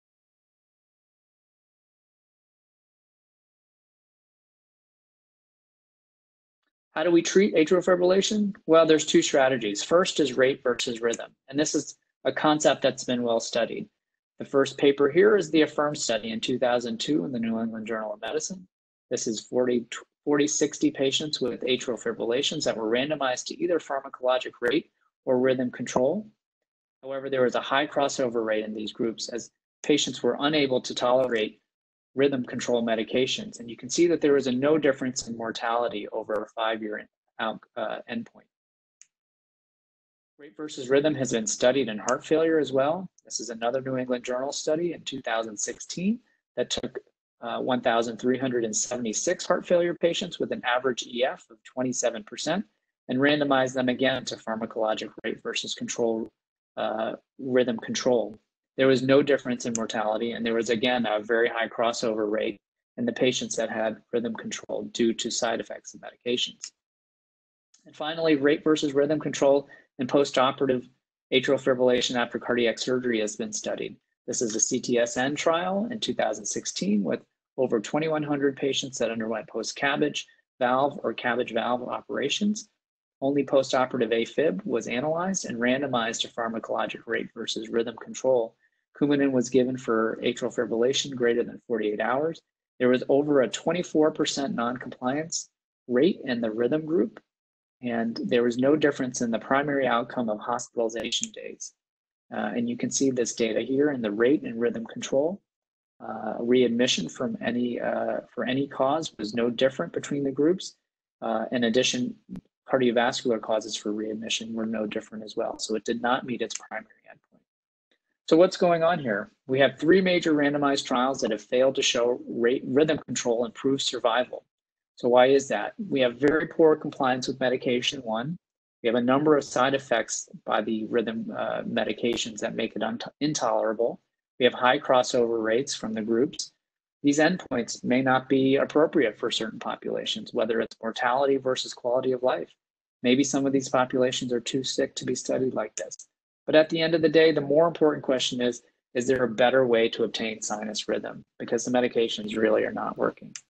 AATS Atrial Fibrillation Mini Theater with Dr. Isaac George: “A Better Way to Obtain NSR”
- Watch Dr. Isaac George as he discusses 3 major clinical trials, all New England Journal of Medicine (NEJM) publications. Among them, CTSN and AFFIRM in which patients were randomized to pharmacologic rate vs. rhythm control. Dr. George concludes that the three major randomized trials that have failed to show rate – rhythm control improves survival. Why? There is very poor compliance with medication. There are also a number of side effects from the rhythm medications that make them intolerable. The important question that needs to be answered is: Is there a better way to obtain sinus rhythm because the medications really are not working?